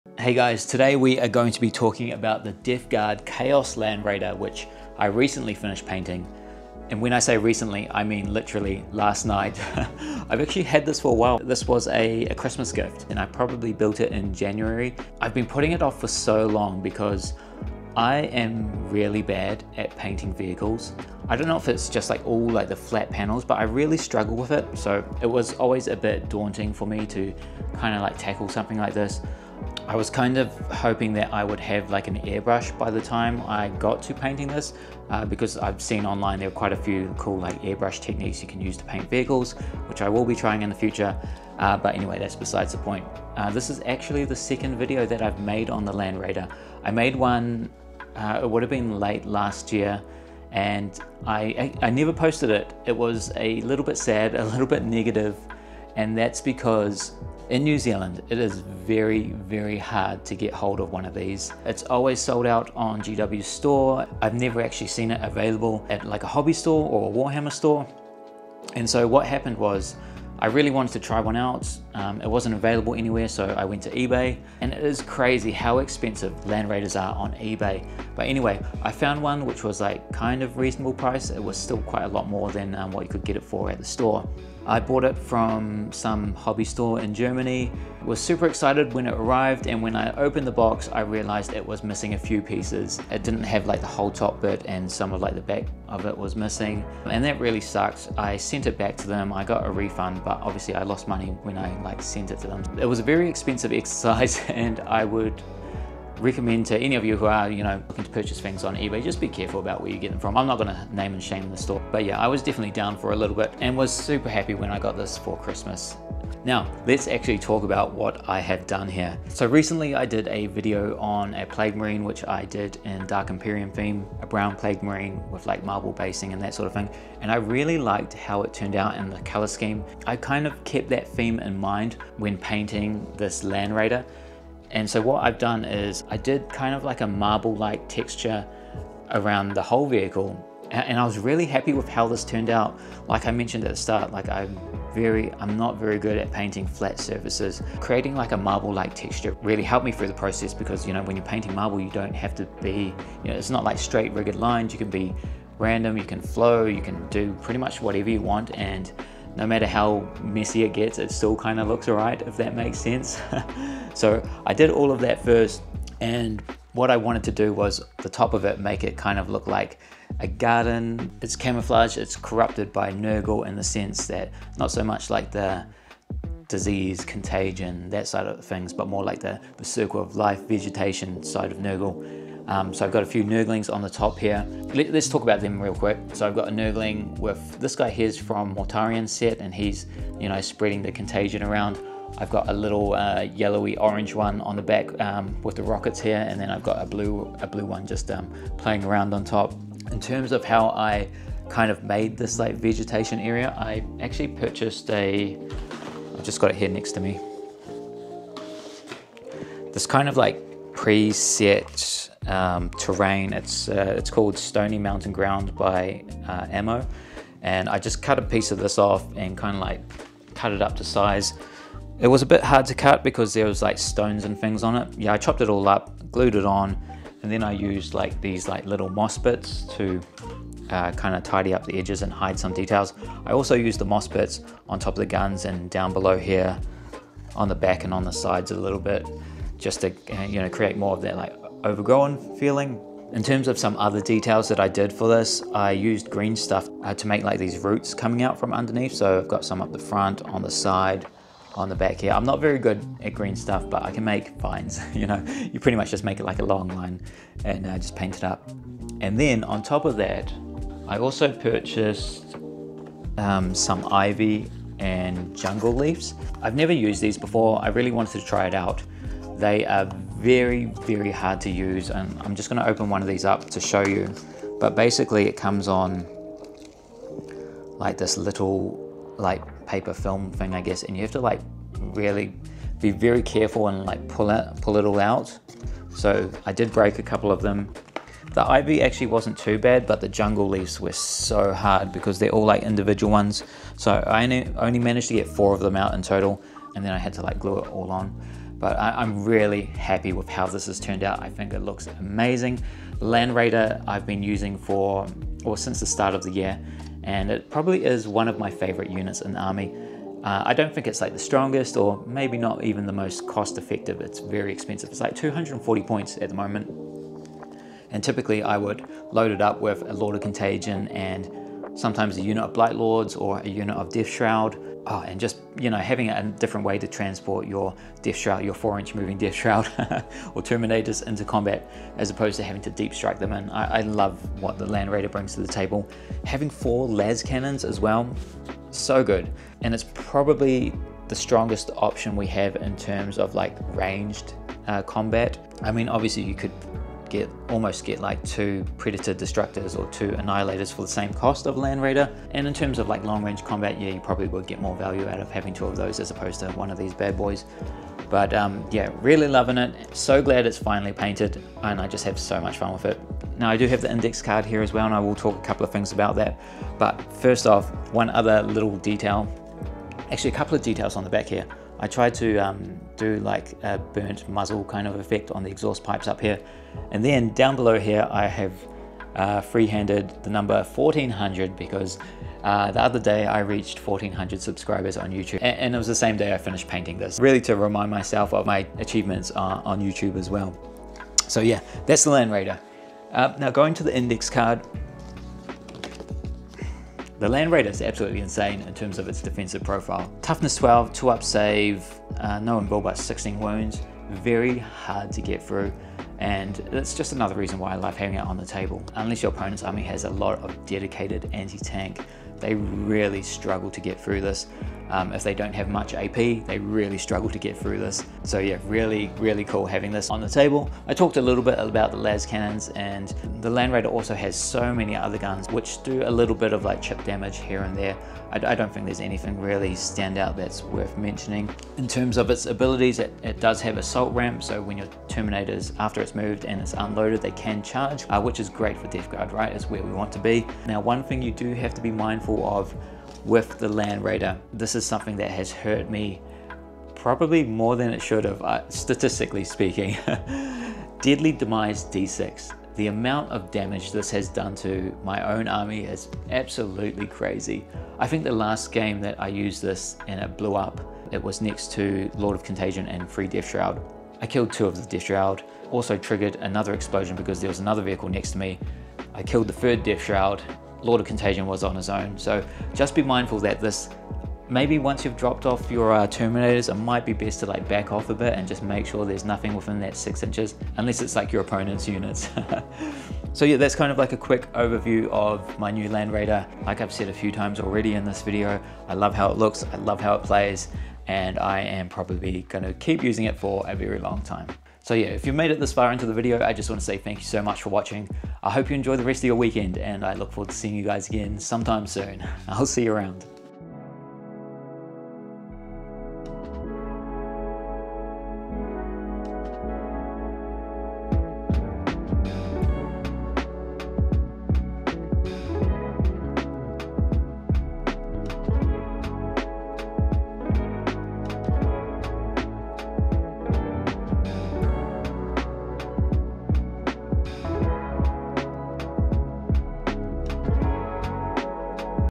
Nurgle's Garden Death Guard Chaos Land Raider Showcase - Warhammer 40k
The chonkiest tank in the Death Guard roster is here and blessed by Grandfather Nurgle himself! My vision for this model was to incorporate the theme of Nurgle's garden and have a few Nurglings playing around. Hope you guys like it!

Similar to the last Plague Marine I painted, I went for a marble-like theme for the tank and am pretty happy with how it turned out. Also used a bit of greenstuff, grass tuffs and vegetation to add some life to it!

On the table, I've been bringing the Land Raider for a while now and love transporting a Lord of Contagion with some Terminators. Having 4 las cannons too is so much fun!